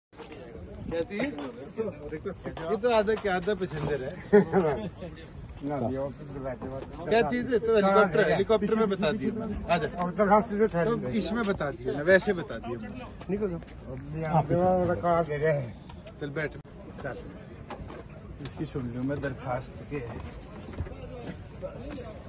जब डिप्टी सीएम दुष्यंत चौटाला ताऊ को ले चले अपने साथ हेलीकॉप्टर में ।
चलो ताऊ जी साथ बतलावेंगे मन मर्जी,
हेलीकॉप्टर में साथ बैठकै सुनाइये अर्जी।